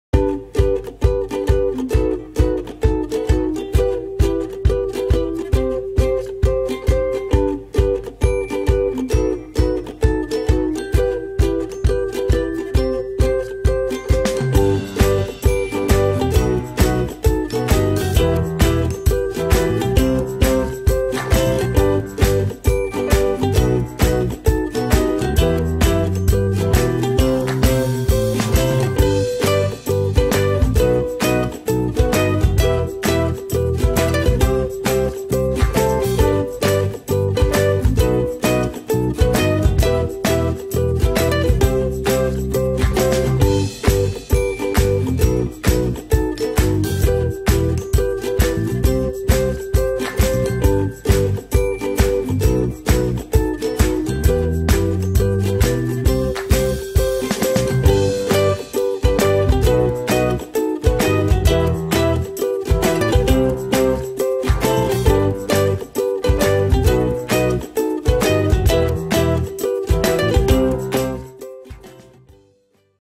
Live Room 1.0
Our augmented reality based e-commerce solution where user can drag and drop the product in their own real life space to get the better understanding how the product looks in their room.